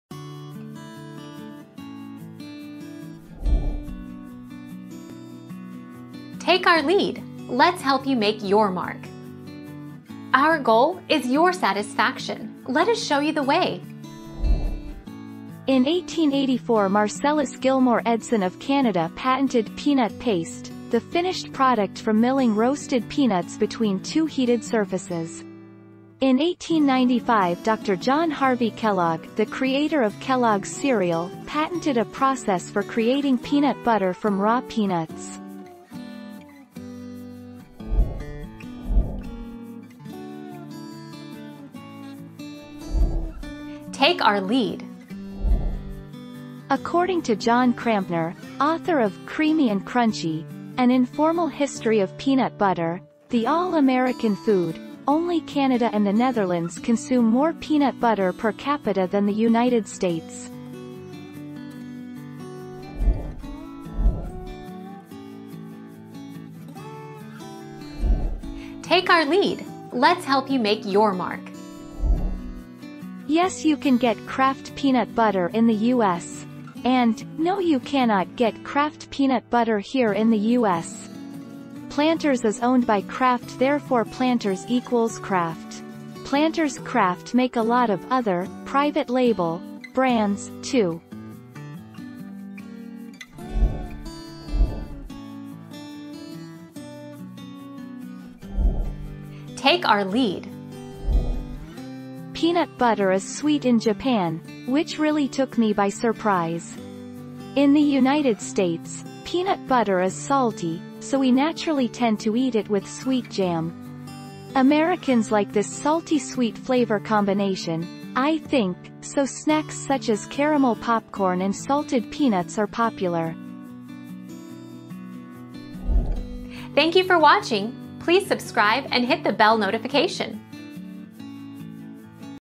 Who invented peanut butter and why?
00:00 - Who invented peanut butter and why?
00:39 - What country eats the most peanut butter?
01:04 - Why is Kraft Peanut Butter not sold in US?
01:35 - Do Japanese eat peanut butter?

Laura S. Harris (2021, January 31.) Who invented peanut butter and why?
    AskAbout.video/articles/Who-invented-peanut-butter-and-why-216286